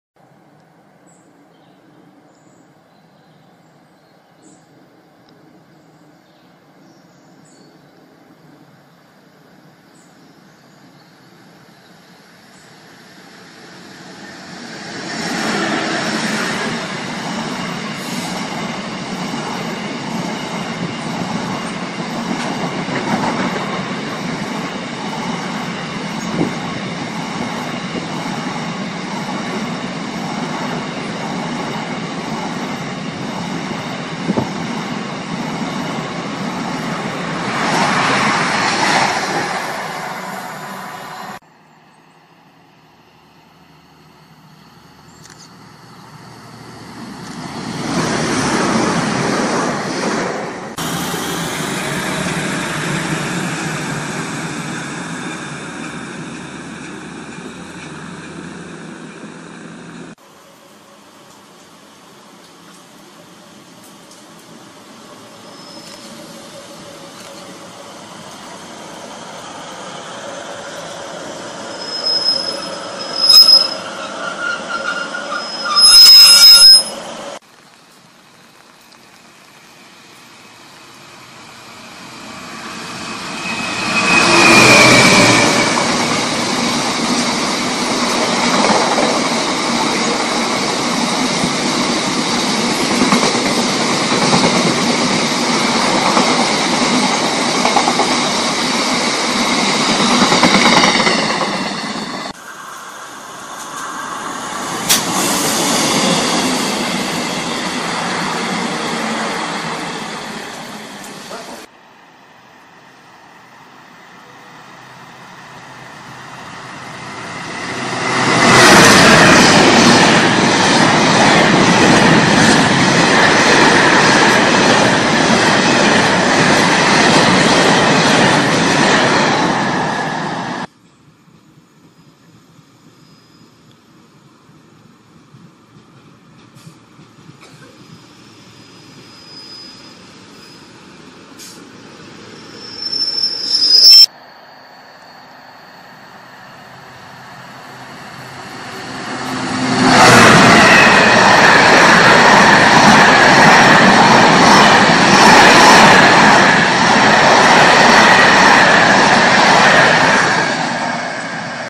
Hope Valley (Sheffield To Manchester Line) Part 1
Filmed during October 2009, whilst sat at Hope Station just outside Castleton, I manage to film quite a few trains arriving and departing from the platforms either side and also manage to catch a glimpse of the freight traffic passing through